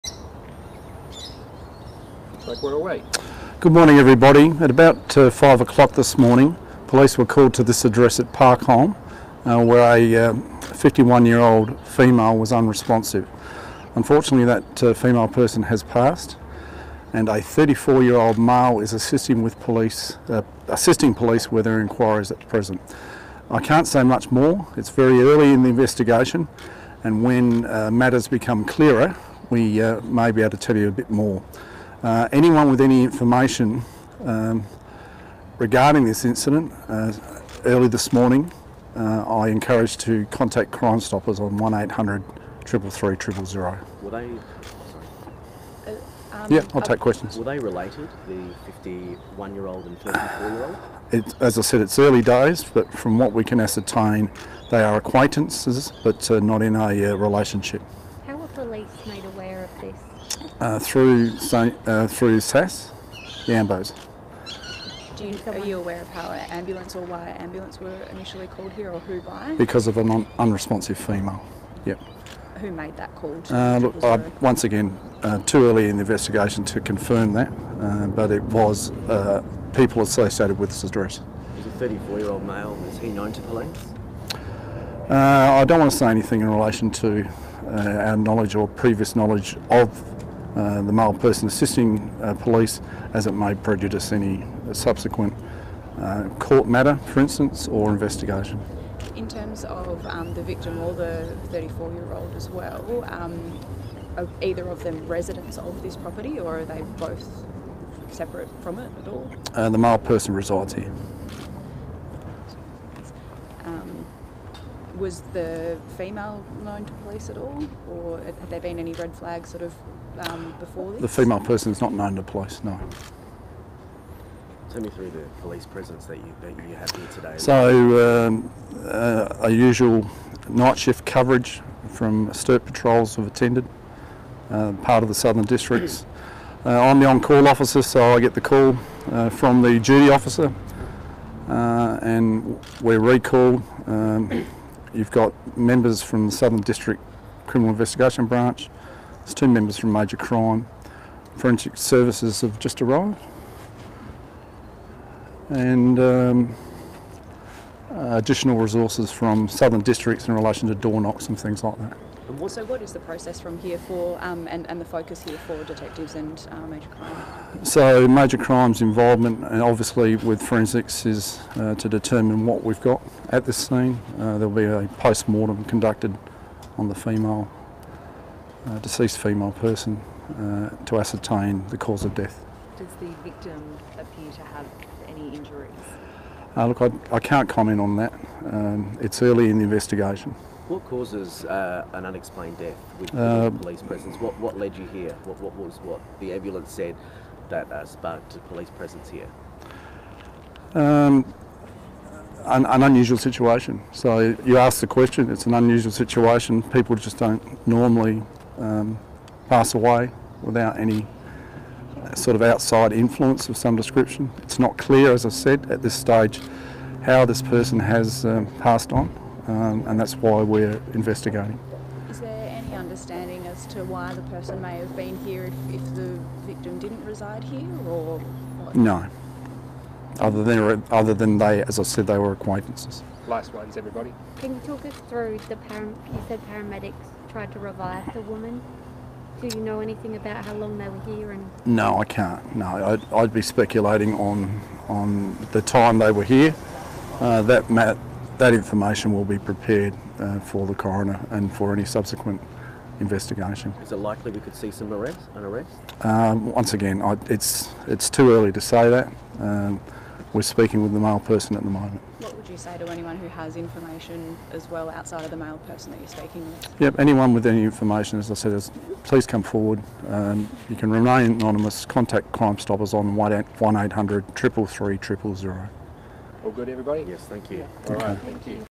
Suspicious death at Park Holme
Detective Chief Inspector Mick Fisher, officer in charge of Southern District Criminal Investigation Branch speaks to the media about the death of a woman at Park Holme. Read more below:

Police and emergency services are currently at a Park Holme address after a person was found unresponsive.
Just before 5am this morning (Tuesday 6 September), police and ambulance crews were called to Condada Avenue, Park Holme after a 51-year-old Glengowrie woman was found collapsed and unresponsive at the premises. Despite attempts at the scene to revive the person, sadly the woman died.A 33-year-old man from the address has been arrested and is being interviewed by Southern District detectives at Christies Beach Police Station.Police remain at the scene and the circumstances surrounding the person’s death are currently under investigation.Anyone with information that can assist police is asked to call Crime Stoppers on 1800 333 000,